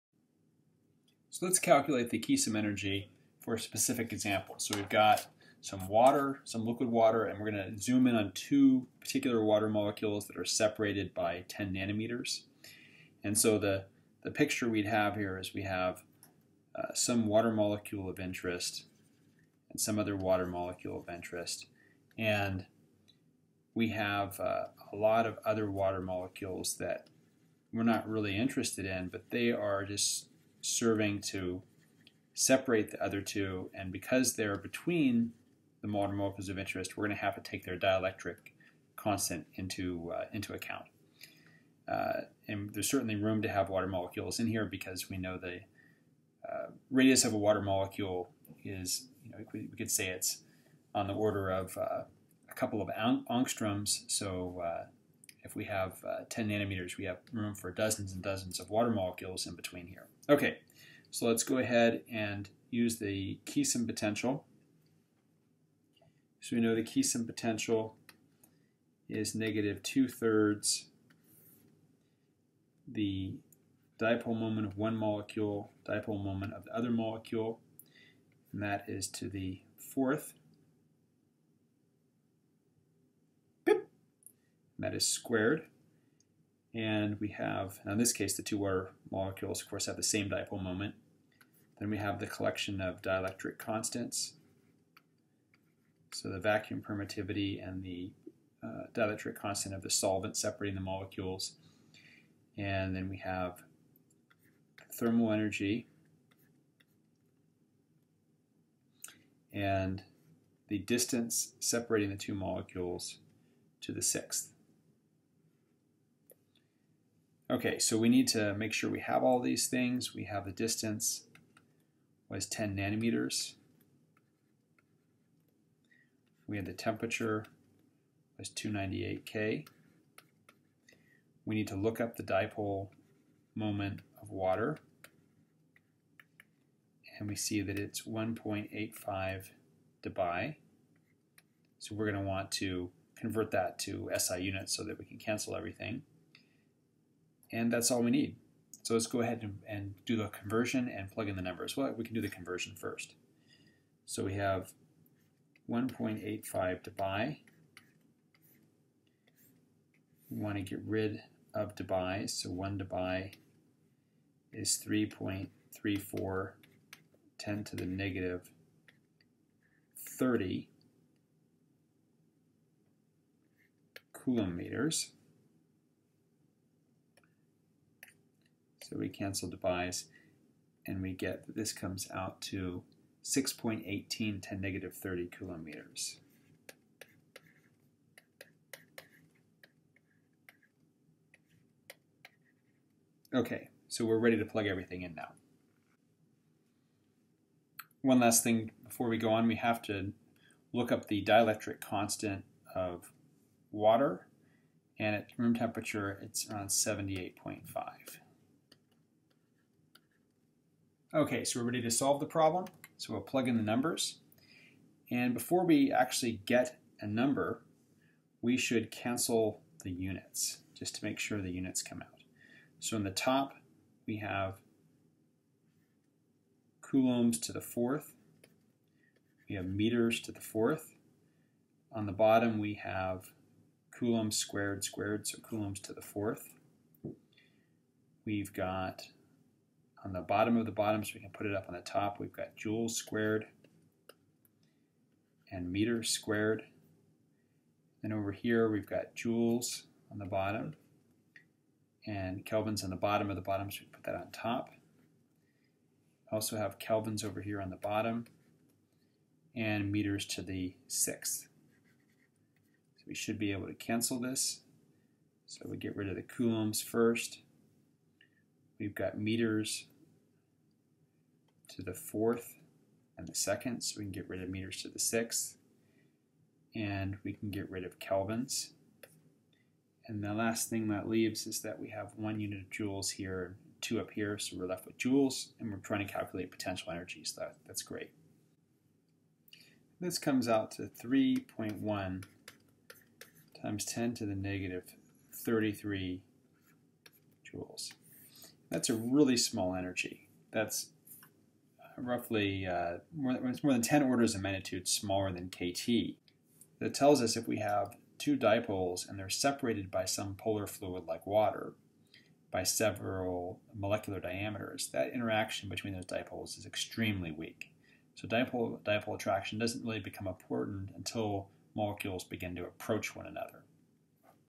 Keesom Example
How strong is the Keesom interaction between a pair of water molecules?